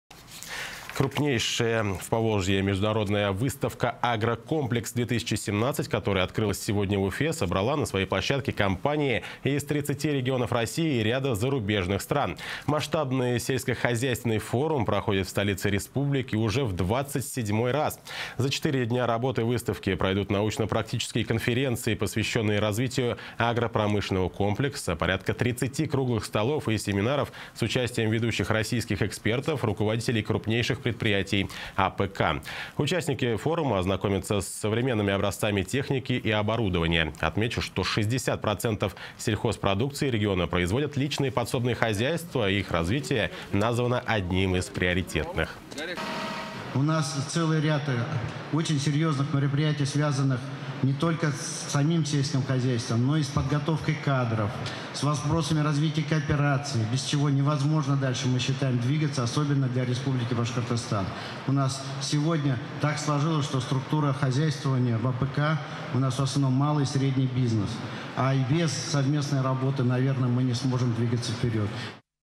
В Уфе открылась международная выставка "АгроКомплекс-2017"
Крупнейшая в Поволжье международная выставка агрокомплекс-2017, которая открылась сегодня в Уфе, собрала на своей площадке компании из 30 регионов России и ряда зарубежных стран. Масштабный сельскохозяйственный форум проходит в столице республики в двадцать седьмой раз.

Комментарий: Ирек Мухаметдинов, заместитель премьер-министра Правительства РБ.